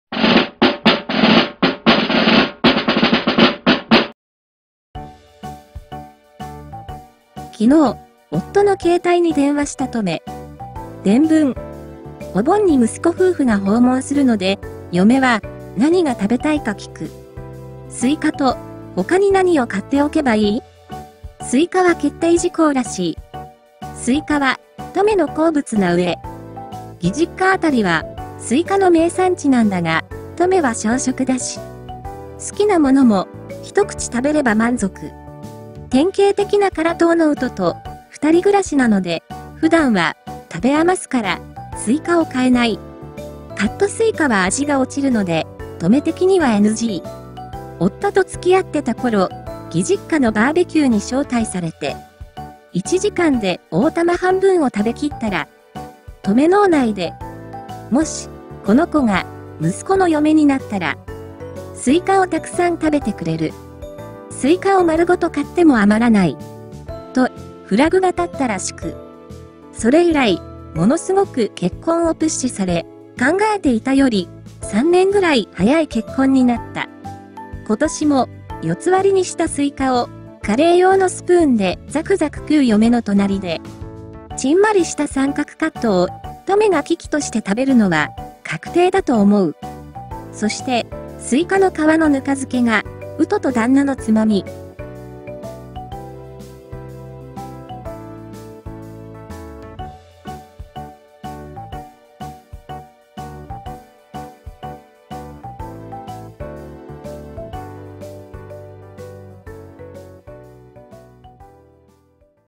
【ほのぼの】トメ「お盆に来るんでしょ？嫁ちゃんはスイカの他に何食べたい？」私（スイカは決定事項なんだｗ）→夫と付き合ってた頃、義実家のバーベキューに招待されて・・
DOVA-SYNDROME